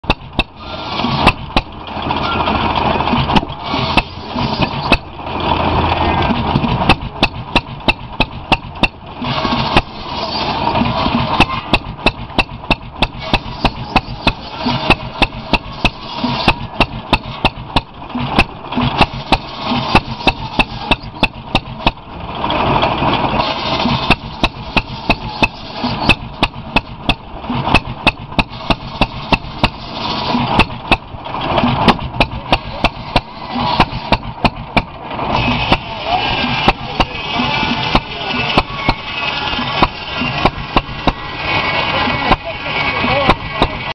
International Buzzsaw at Chemung County Old Timers 2016 Show
Buzzsaw run by Randy B.  At Chemung County Old Timers Association 2016 show.